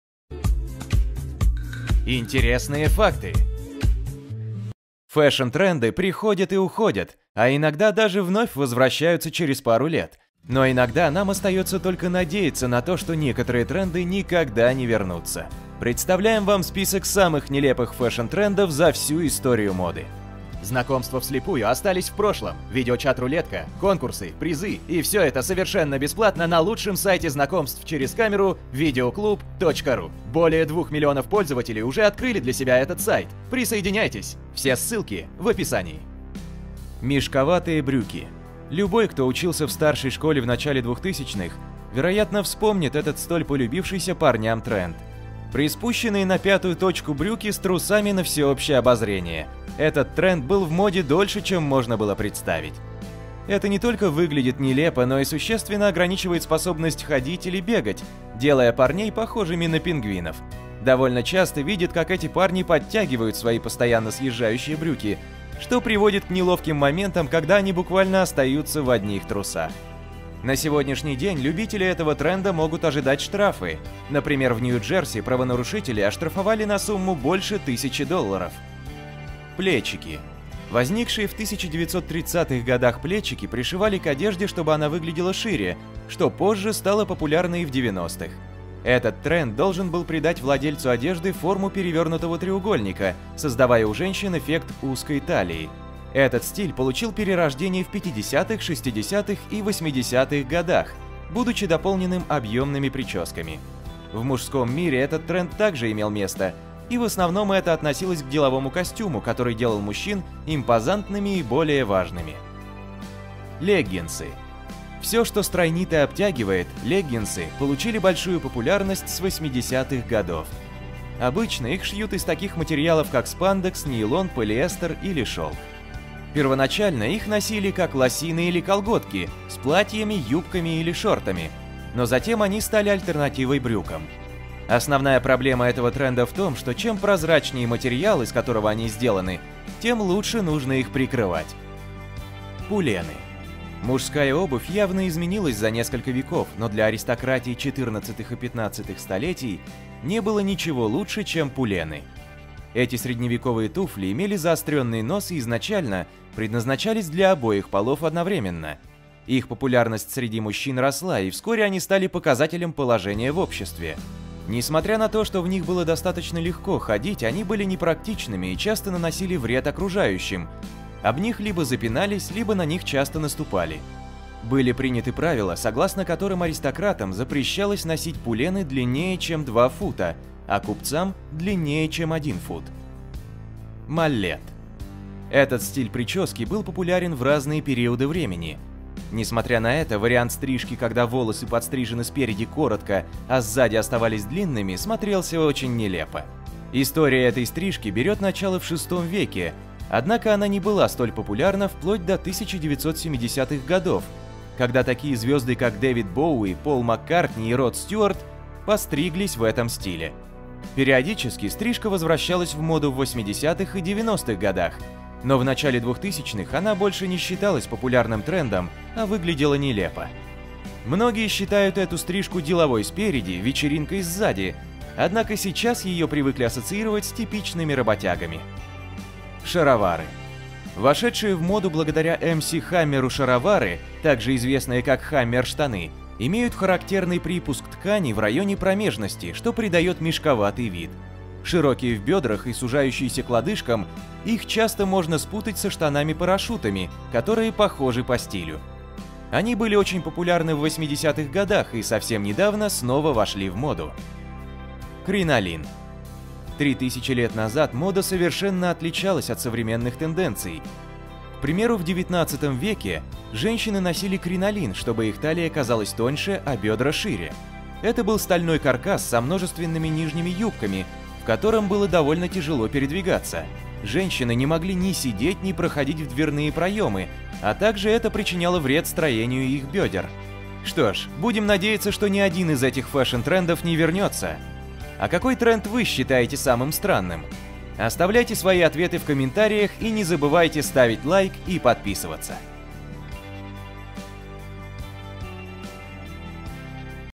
ТОП 10 УЖАСНЫХ FASHION ТРЕНДОВ О КОТОРЫХ ХОЧЕТСЯ ЗАБЫТЬ
Фэшн тренды приходят и уходят, а иногда даже вновь возвращаются через пару лет
Но иногда нам остается только надеяться на то, что некоторые тренды никогда не вернутся.
Представляем вам список самых нелепых фэшн трендов за всю историю моды.

10) Мешковатые брюки
Любой, кто учился в старшей школе в начале 2000-х вероятно вспомнит этот тренд столь полюбившейся парням.
Приспущенные на пятую точку брюки с трусами на всеобщее обозрение: этот тренд был в моде дольше чем можно было представить.
Это не только выглядит нелепо, но и существенно ограничивает способность ходить или бегать, делая парней похожими на пингвинов.
Довольно часто видят, как эти парни подтягивают свои постоянно съезжающие брюки, что приводит к неловким моментам, когда они буквально остаются в одних трусах.
На сегодняшний день любителей этого тренда могут ожидать штрафы.
Например, в Нью Джерси правонарушителей оштрафовали на сумму больше 1000 $.

9) Плечики
Возникшие в 1930-х гг. плечики пришивали к одежде, чтобы она выглядела шире, что позже стало популярно и в 90-х.
Этот тренд должен был придать владельцу одежды форму перевернутого треугольника, создавая у женщин эффект узкой талии.
Этот стиль получил свое перерождение в 50-х, 60-х и 80-х гг., будучи дополненным объемными прическами.
В мужском мире этот тренд также имел место, и в основном это относилось к деловому костюму, который делал мужчин импозантными и более важными.

8) Леггинсы
Все, что стройнит и обтягивает- леггинсы получили большую популярность с 80-х гг.
Обычно их шьют из таких материалов как спандекс, нейлон, полиэстер или шелк.
Первоначально, их носили как лосины или колготки с платьями, юбками или шортами, но затем они стали альтернативой брюкам.
Основная проблема этого тренда в том, что чем прозрачнее материал, из которого они сделаны тем лучше нужно их прикрывать.

7) Пулены
Мужская обувь явно изменилась за несколько веков, но для аристократии 14-х и 15-х столетий не было ничего лучше, чем пулены.
Эти средневековые туфли имели заостренный нос и изначально предназначались для обоих полов одновременно.
Их популярность среди мужчин росла и вскоре они стали показателем положения в обществе. Несмотря на то, что в них было достаточно легко ходить, они были непрактичными и часто наносили вред окружающим: об них либо запинались, либо на них часто наступали.
Были приняты правила, согласно которым аристократам запрещалось носить пулены длиннее чем 2 фута, а купцам – длиннее чем один фут.

6) Маллет
Этот стиль прически был популярен в разные периоды времени. Несмотря на это, вариант стрижки, когда волосы пострижены спереди коротко, а сзади оставались длинными, смотрелся очень нелепо. История этой стрижки берет начало в 6 веке, однако, она не была столь популярна вплоть до 1970-х гг., когда такие звезды как Дэвид Боуи, Пол Маккартни и Род Стюарт подстриглись в этом стиле.
Периодически стрижка возвращалась в моду в 80-х и 90-х гг., но в начале 2000-х она больше не считалась популярным трендом, а выглядела нелепо.
Многие считают эту стрижку «деловой спереди, вечеринкой сзади», однако сейчас ее привыкли ассоциировать с типичными работягами.

5) Шаровары
Вошедшие в моду благодаря МС Хаммеру, шаровары также известные как хаммер штаны имеют характерный припуск ткани в районе промежности, что придает мешковатый вид.
Широкие в бёдрах и сужающиеся к лодыжкам- их часто можно спутать со штанами парашютами, которые похожи по стилю.
Они были очень популярны в 80-х гг., и совсем недавно снова вошли в моду.

4) Кринолин
3 тысячи лет назад, мода совершенно отличалась от современных тенденций.
К примеру, в 1800-х гг. женщины носили кринолин, чтобы их талия казалась тоньше, а бедра- шире.
Это был стальной каркас со множественными нижними юбками, в котором было довольно тяжело передвигаться. Женщины не могли ни сидеть, ни проходить в дверные проемы, а также это причиняло вред строению их бедер.

Что ж, будем надеяться, что ни один из этих фэшн трендов не вернется. А какой тренд вы считаете самым странным?